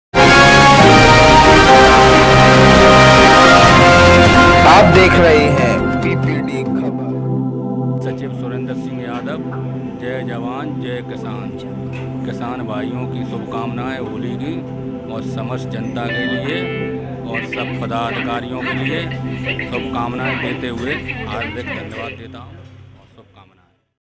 etah। किसान नेता सुरेन्द्र कुमार की ओर से सभी देशवासियों को होली की हार्दिक शुभकामनाएं
surendra kumar yadav hotel nerlkanthh etah